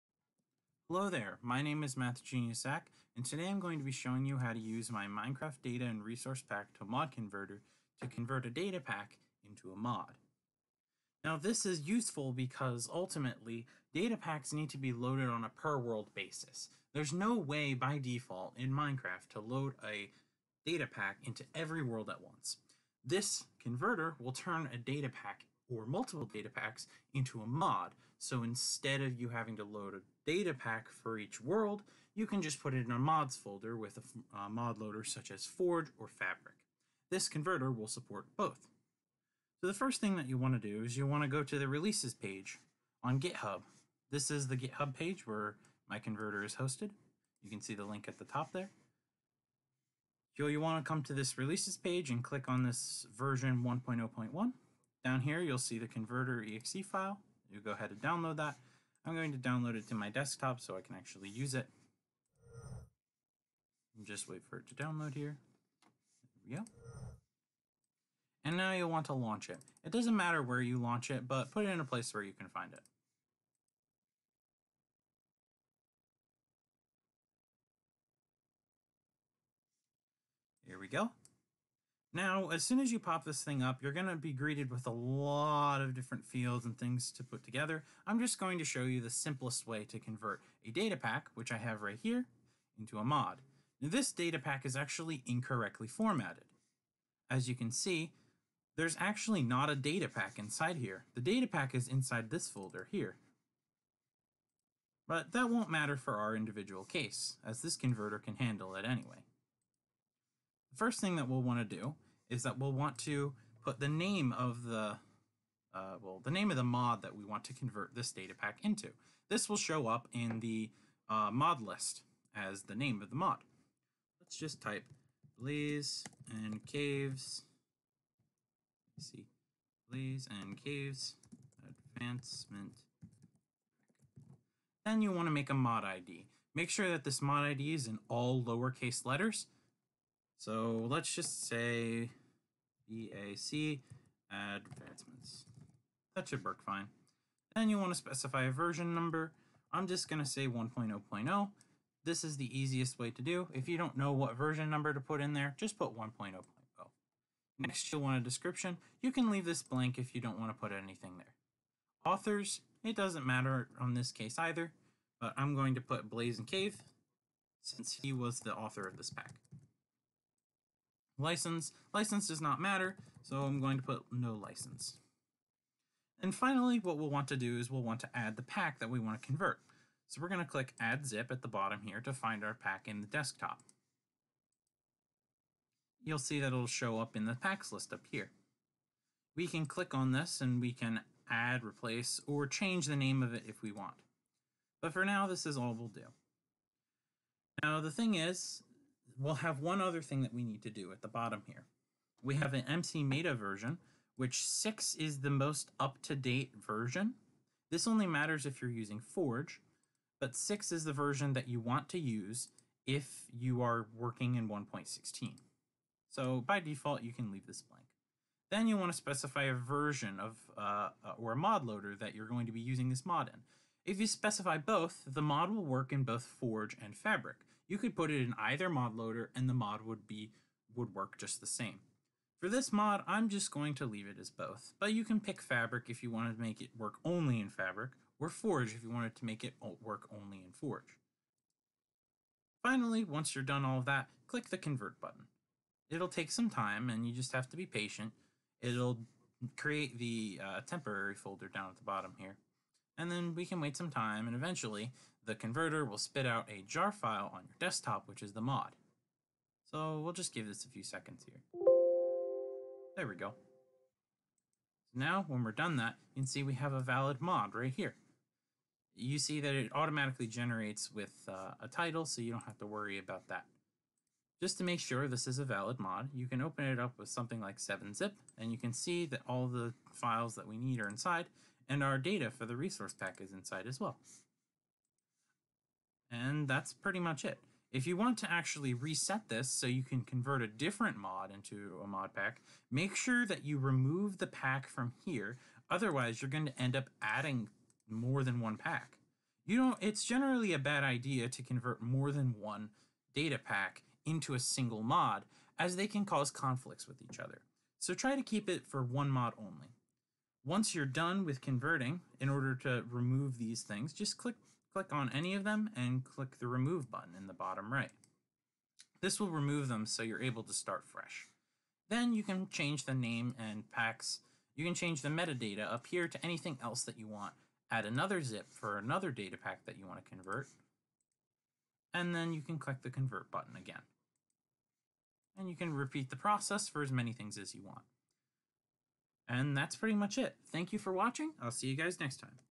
How to convert a Minecraft Datapack into a Mod
mdrpmc (and it's successor, zdpack has been discontinued. For more information on why and alternative solutions,